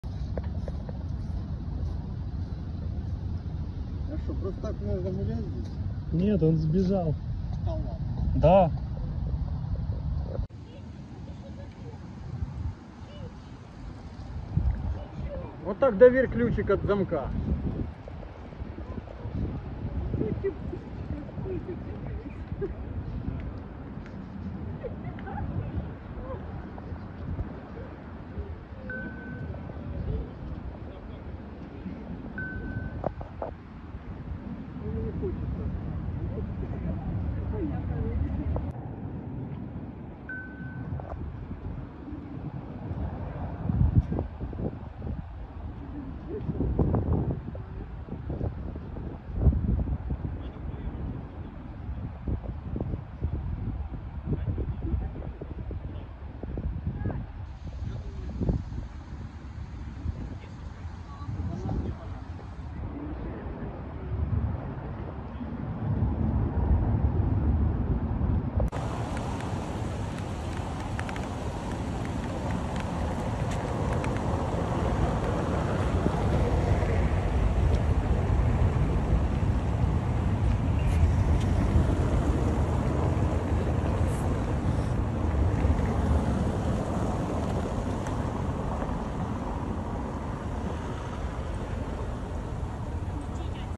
شاهد: شمبانزي هرب من حديقة الحيوانات في خاركيف الأوكرانية فكيف عاد ولقاء ماذا؟
قام الحيوان المعروف باسم تشيشي بفك بوابة السياج الواقي وفتح الأقفال بسهولة، وذهب في نزهة عبر أرجاء المدينة.